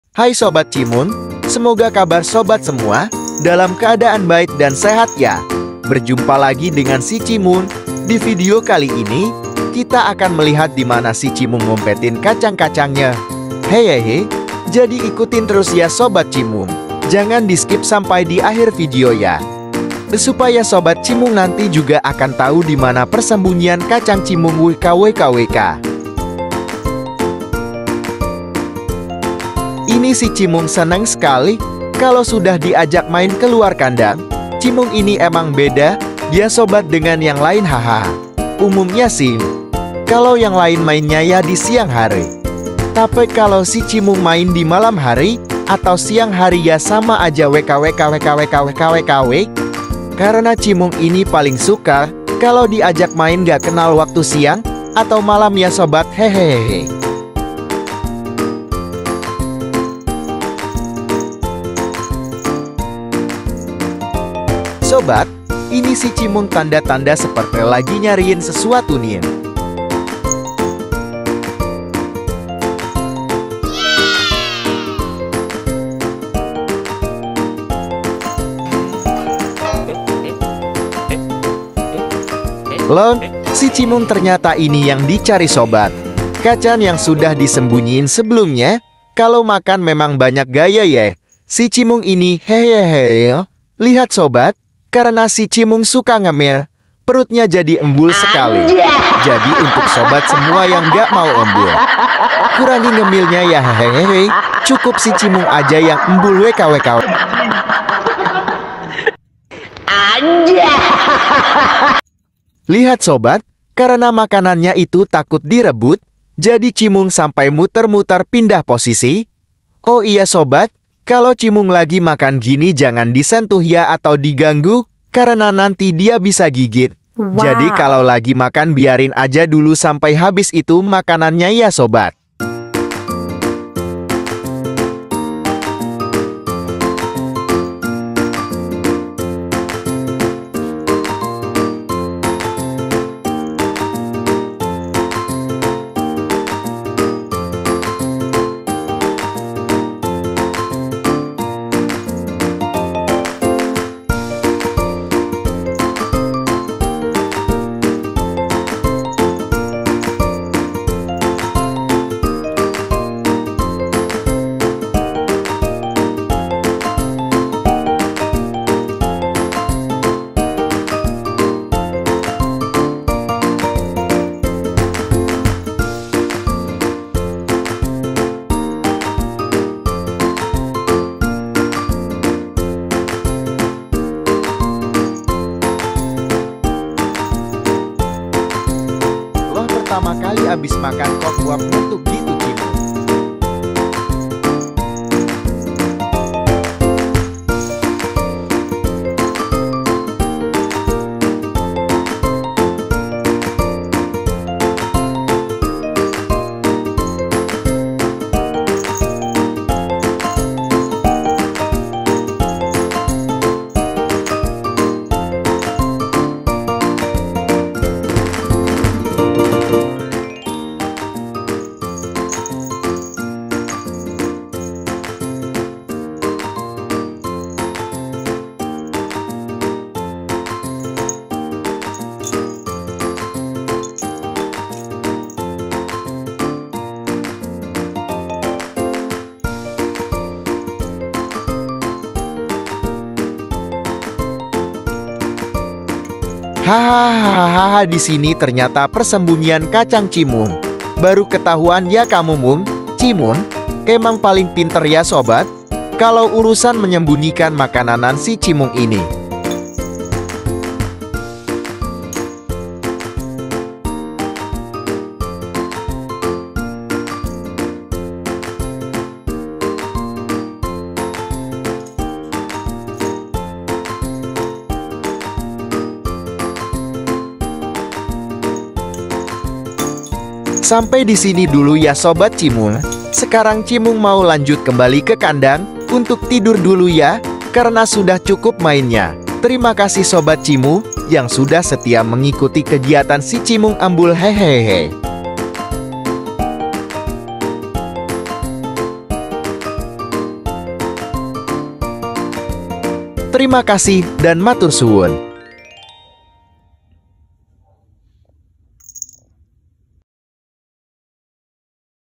Terkuak ‼️😱😱 tempat persembunyian kacang 🥜 cimung 🤣
Hai sobat cimung , vidio kali ini kita akan melihat dimana cimung sembunyikan kacang kacangnya